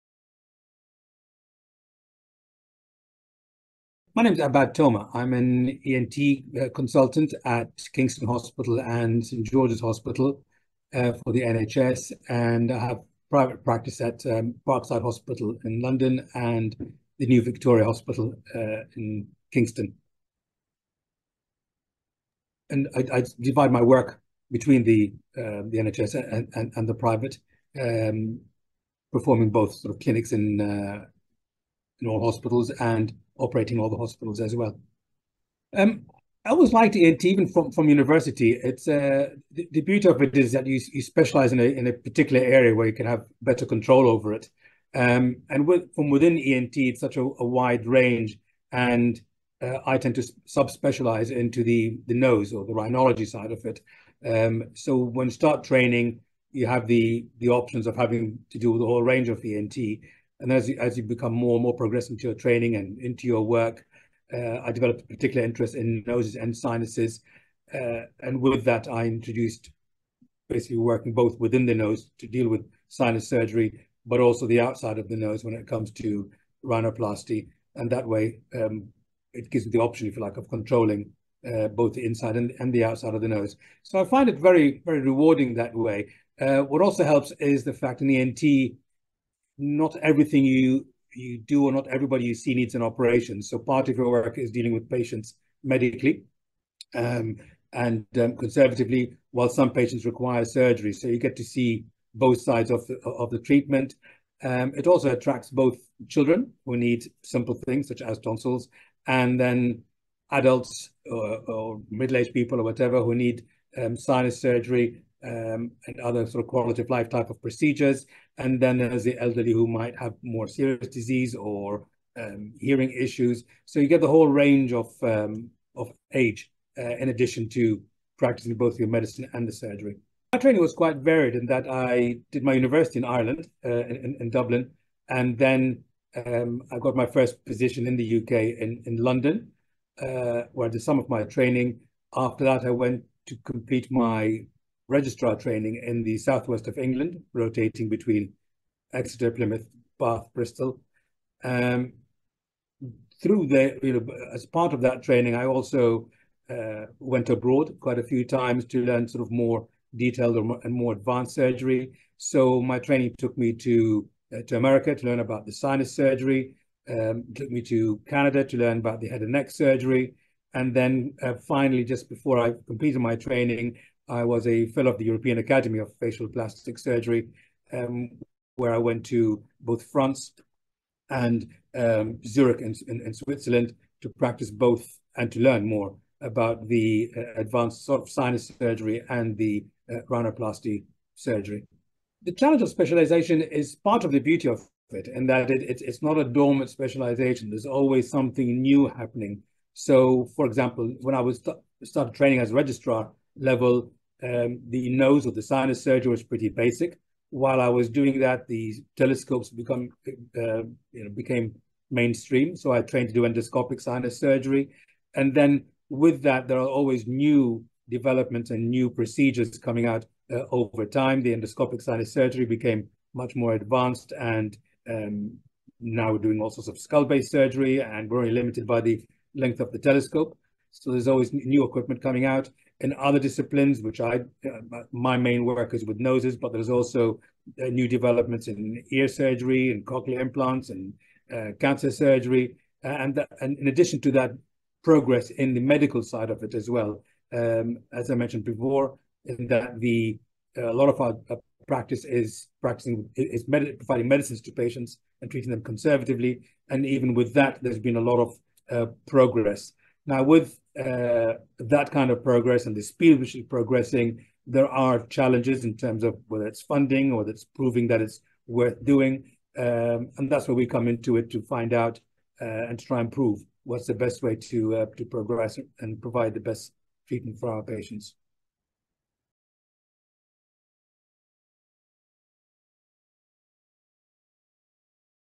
An introduction to Mr Abbad Toma
In this brief introductory video, Mr Abbad Toma, a highly esteemed consultant ENT and plastic surgeon, tells us what the most challenging aspect of his job is, and why he decided to specialise in his specific field.

To consult with Mr Toma, visit his Top Doctors profile here today